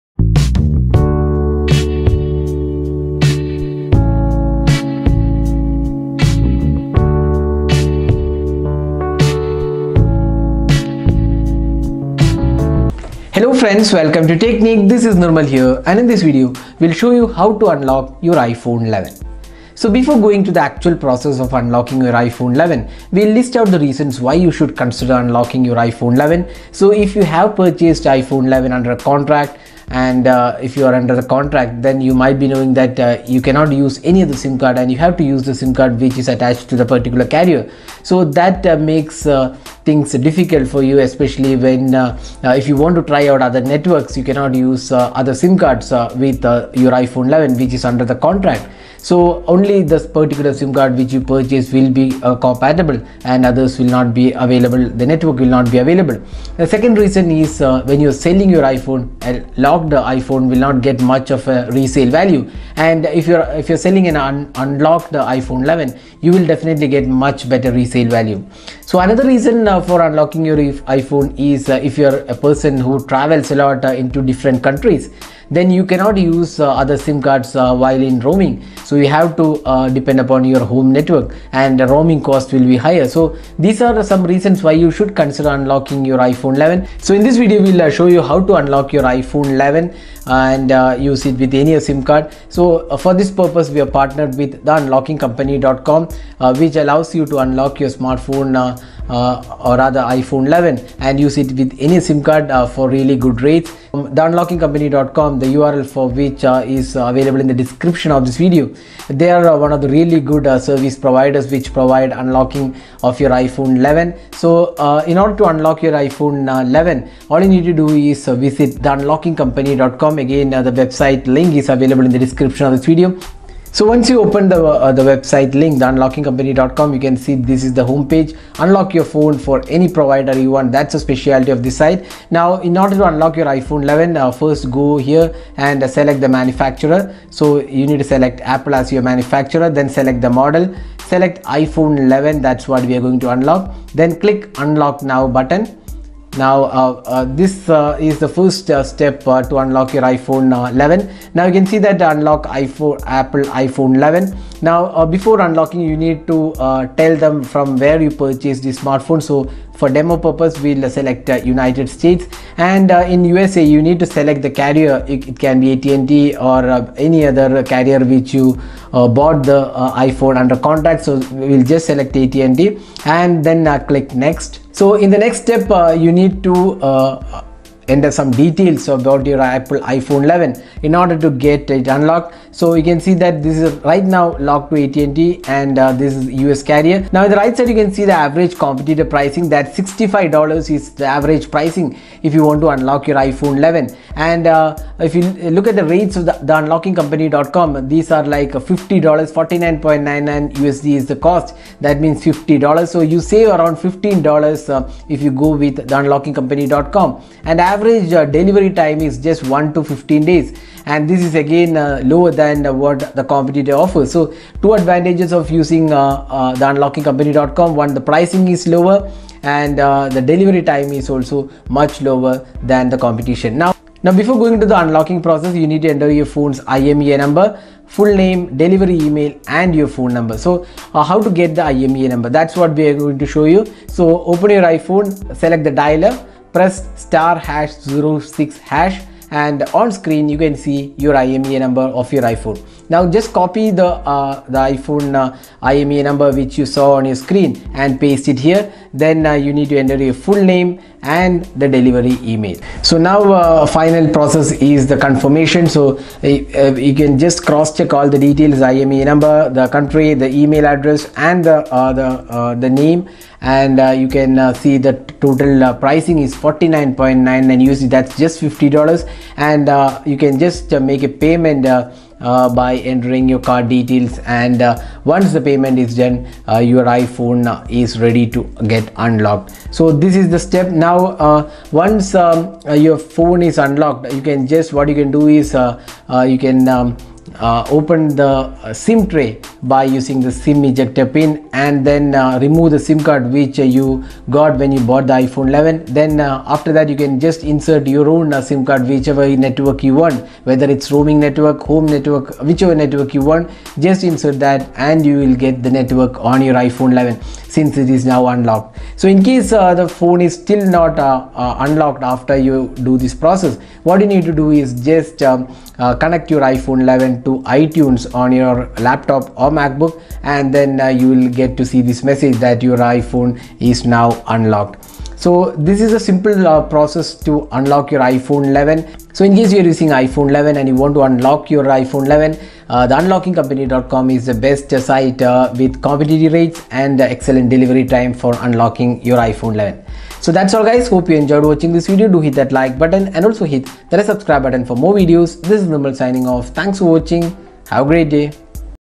How to Unlock iPhone 11 and Use it with Any Carrier
If you want to unlock your iPhone 11 and want to use it with other carriers, then here is how to do it.

How to Unlock iPhone 11 without Jailbreak.